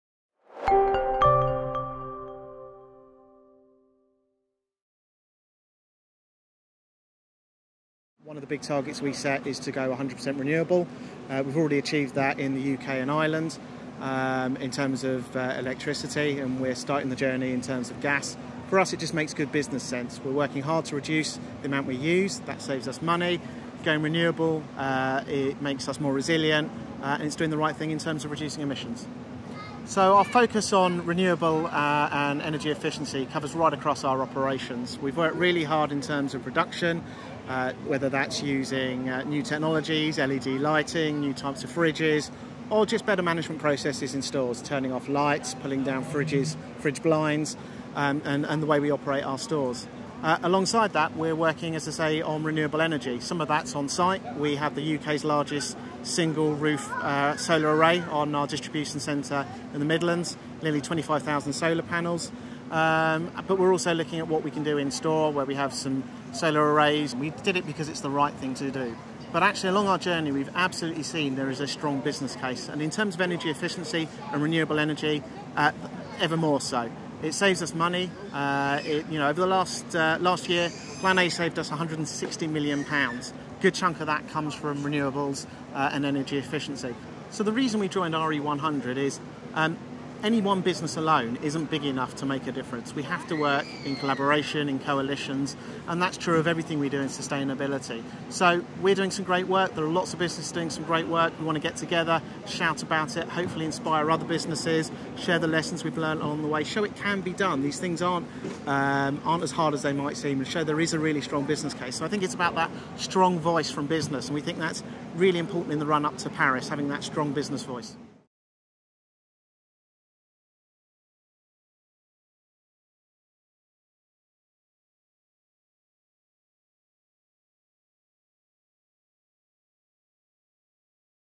Adam Elman, Marks & Spencer’s Head of Global Plan A Delivery
“One business alone isn’t big enough to make a difference, we have to work in collaboration, in coalition, like all things in sustainability. We want to get together, shout about it, hopefully inspire other businesses, share the lessons we have learnt along the way and show it can be done. These things aren’t as hard as they might seem.”

M&S is the first major retailer in the world to have carbon neutral operations and aims to achieve its 100% renewable power goal by 2020 as part of its ‘Plan A’ sustainability strategy.